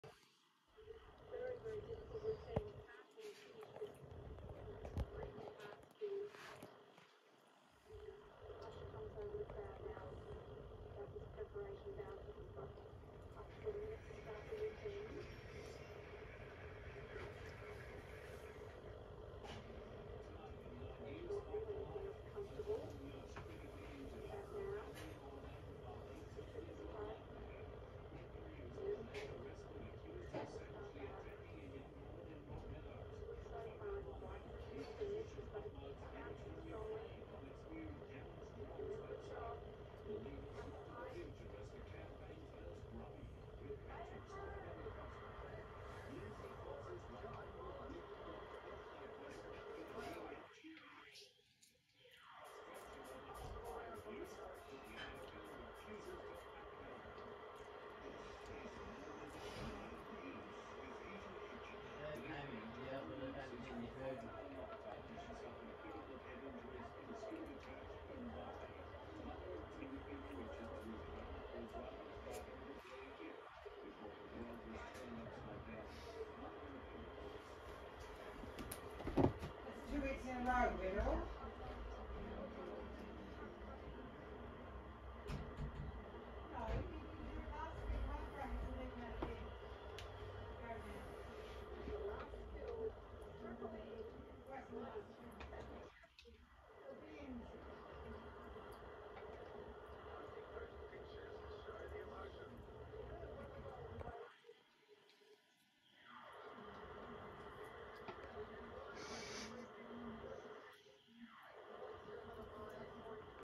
2000 Olympic Trampoline silver routine Ji Wallace
Ji Wallace's silver medal routine from the 2000 Sydney Olympic games from an old VHS tape I found.